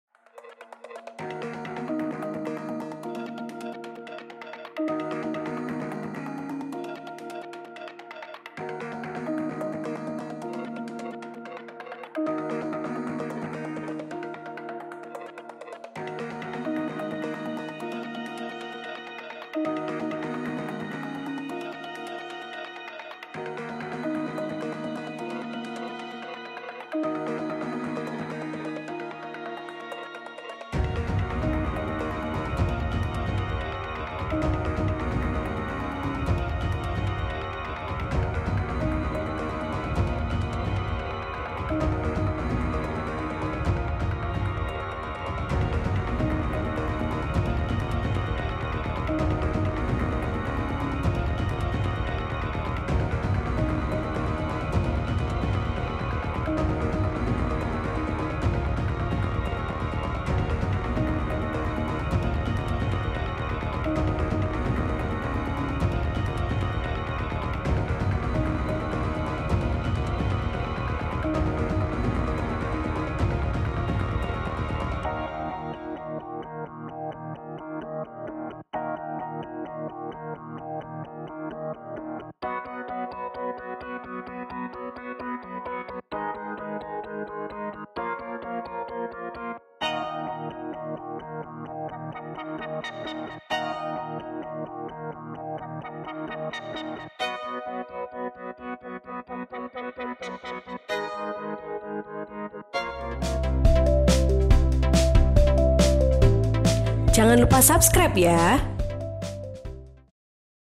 Bocah Terlindas Mobil di Kembangan Terekam CCTV
Korban berinisial AC masih berusia lima tahun, sedangkan pengendara mobil berinisial ZA (44).